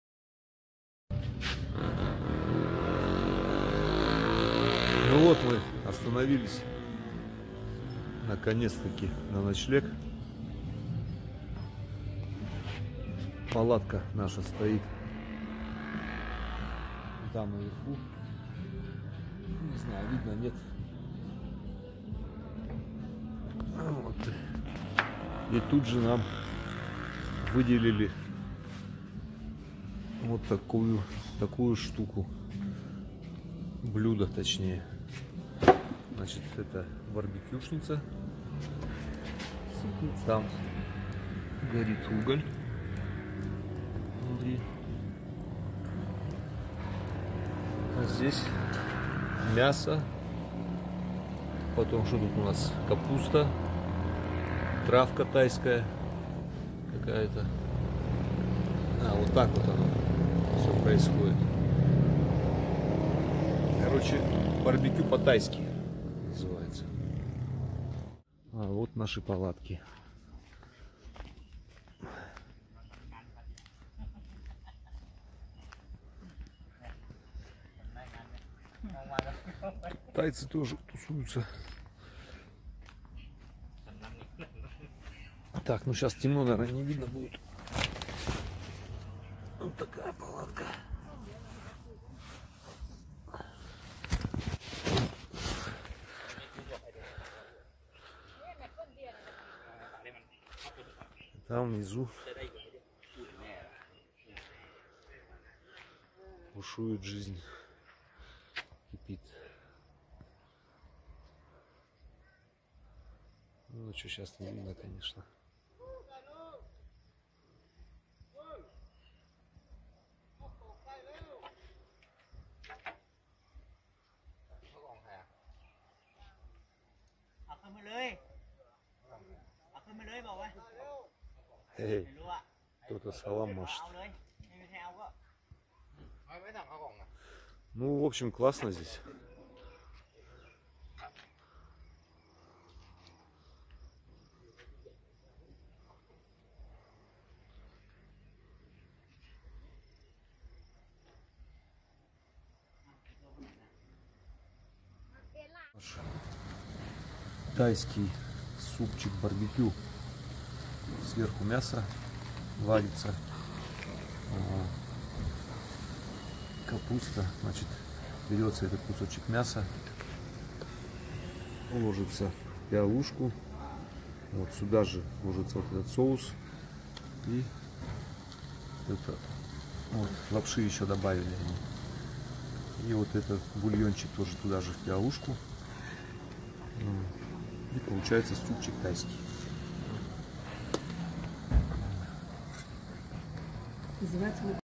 Таиланд | Тайский супчик где-то в горах на севере Таиланда!👍
Таиланд 2021 | Сегодня мы остановились в кемпинге на самой высокой горе в провинции Пхетчабун.  Сняли палатку 800 бат и ужин 200 бат с человека.
 На ужин нам подали суп - барбекю по тайски.